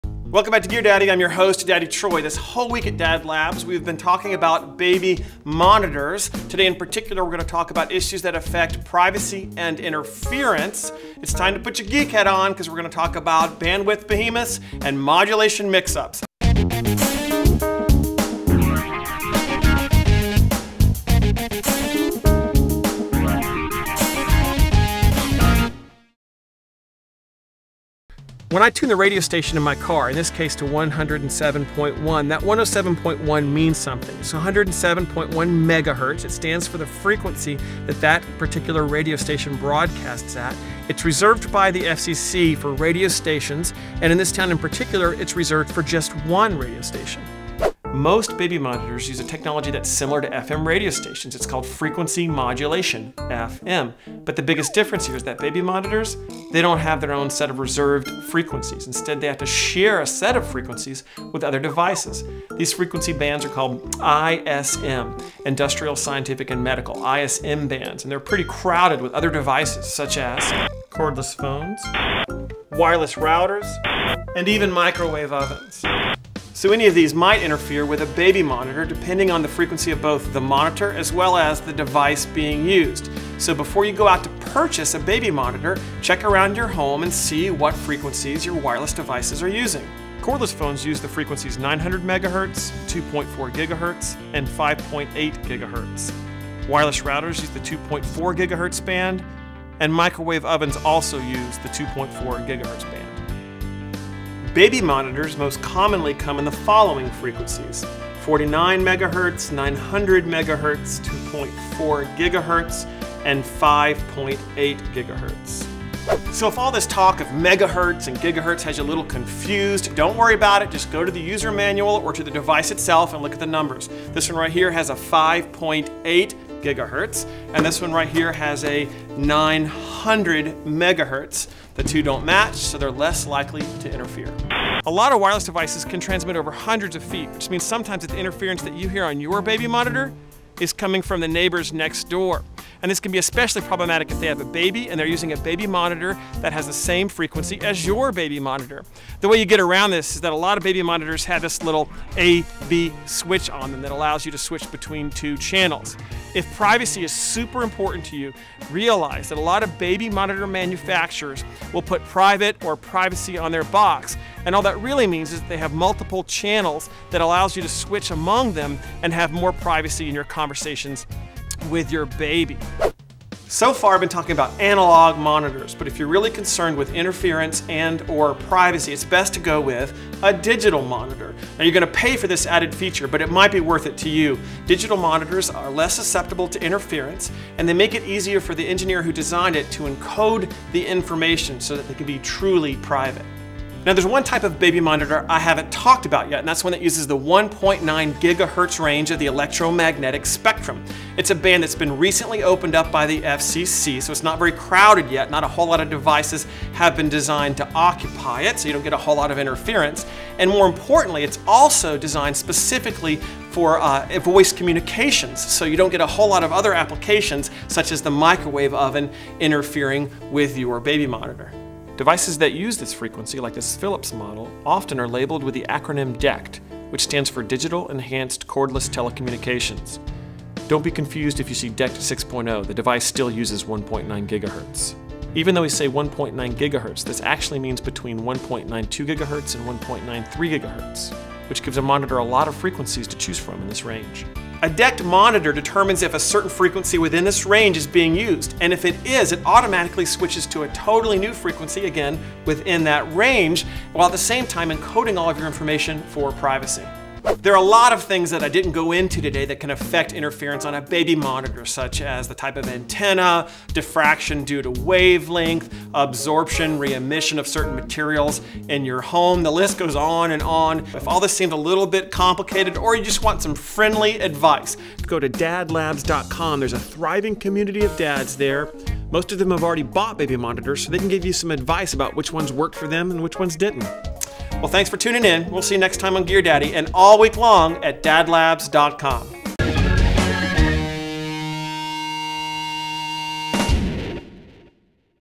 Baby Monitor Tech: Static Causes and Cures - Video
- The task of choosing which baby monitor to select can be a daunting one for dads.  In this episode of Gear Daddy, Daddy Troy clears up the confusion and explains the science and technology behind baby monitors.  What does Megahertz and Gigahertz mean?  What about ISM and DECT?  What wireless technology in your home do these baby products share frequencies with?  Will a digital baby monitor provide more privacy and less interference?  Gear Daddy has the answers to these and other questions about all kinds of baby gear.  DadLabs Ep. 421 Distributed by Tubemogul.